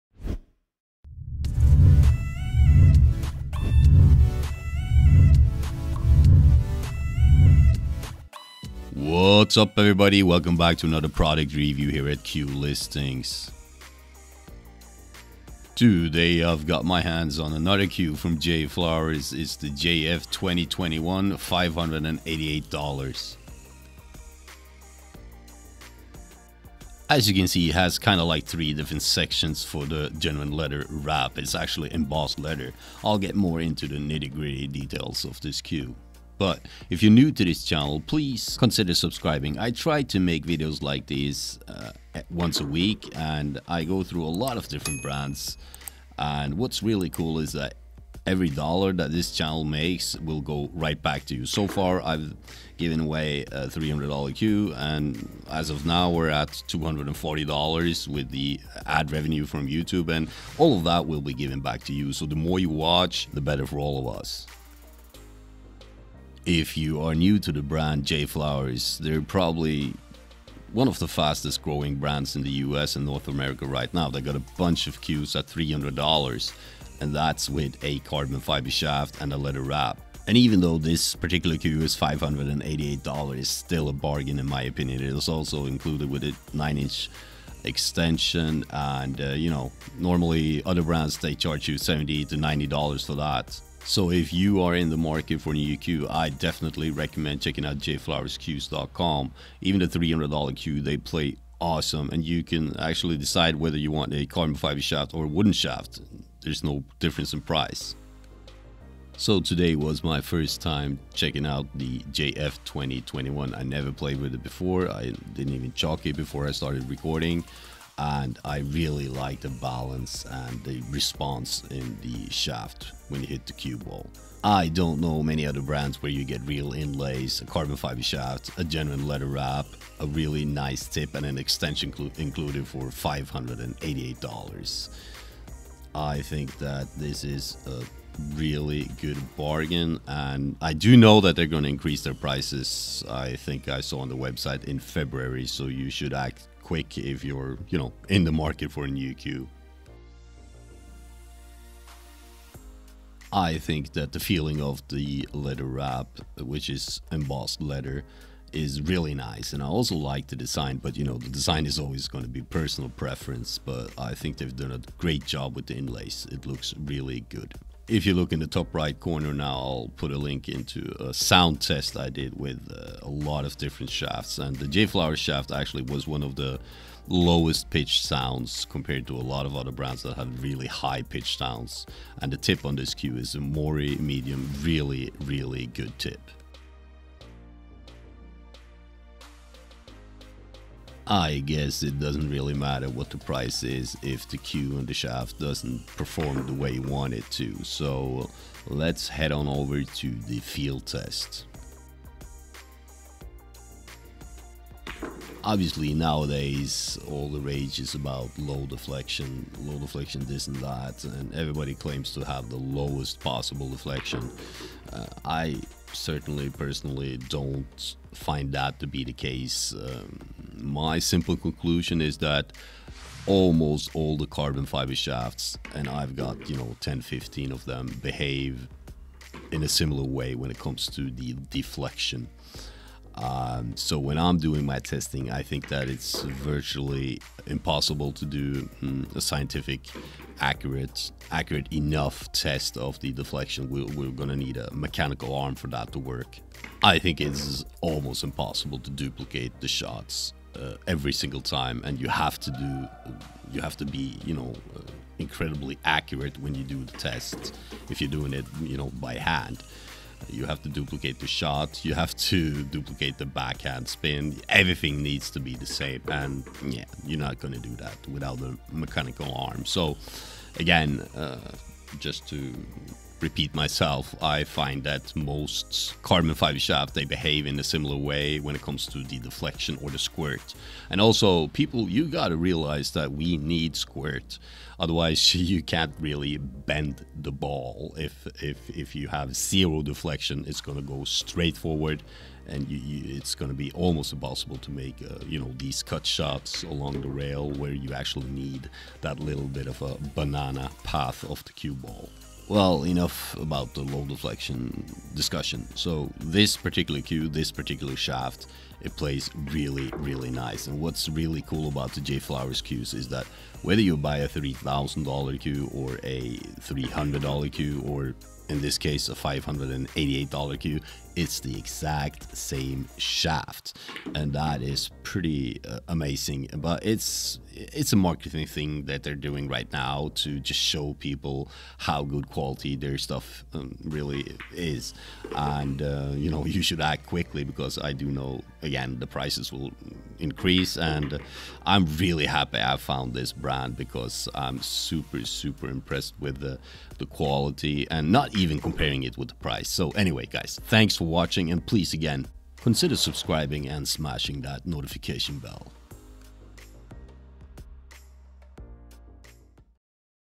JFlowers JF20-21 Cue Review
I've tested the JFlowers JF20-21 cue. In this cue review, I test the JFlowers carbon fiber shaft and give my honest opinion on the quality and price of this pool cue. It comes with a carbon fiber shaft, joint protectors, a genuine leather wrap, and an extension.

00:00 Intro
00:20 JFlowers JF20-21 Pool Cue Price
01:05 JFlowers JF20-21 Pool Cue Review
01:22 JFlowers JF20-21 Pool Cue Extension
02:47 JFlowers JF20-21 Pool Cue Carbon Fiber Shaft
05:05 JFlowers JF20-21 Pool Cue Test Conclusion